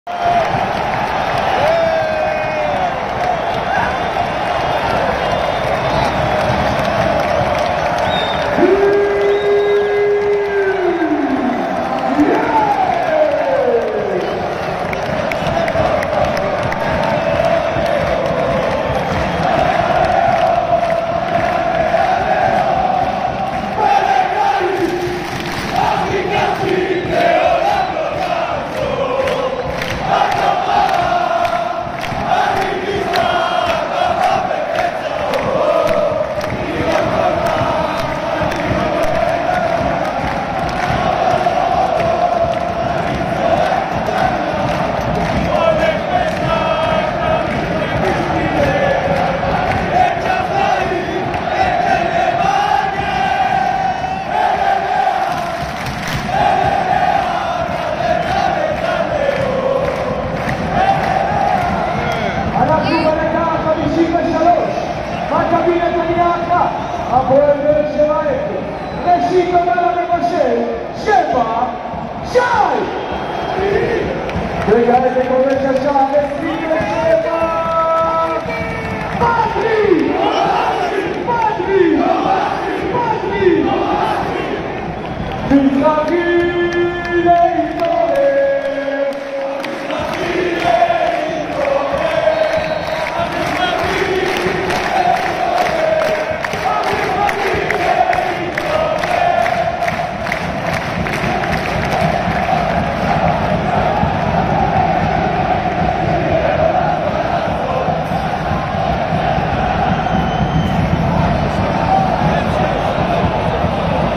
ברגעים הכי קשים-אוהדי נתניה מרעידים את היציעים במשחק מול באר שבע
אוהדי מכבי נתניה חוגגים לאחר השער הראשון של פטריק טוומאסי בניצחון על הפועל באר שבע 0-3